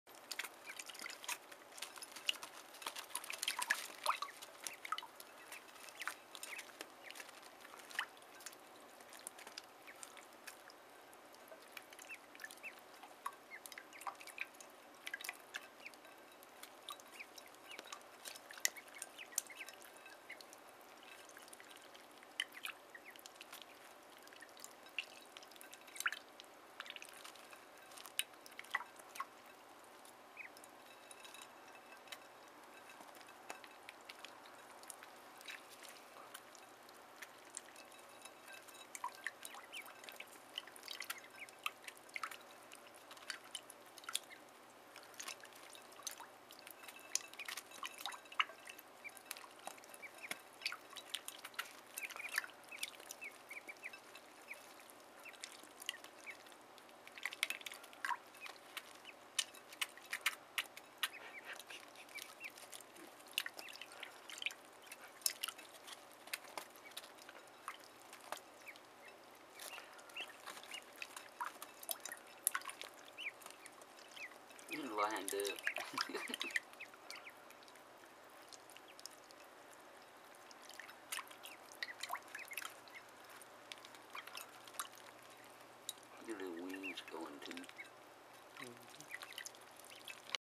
Pool Day 3.AVI
The Quackerettes first introduction to a wading pool.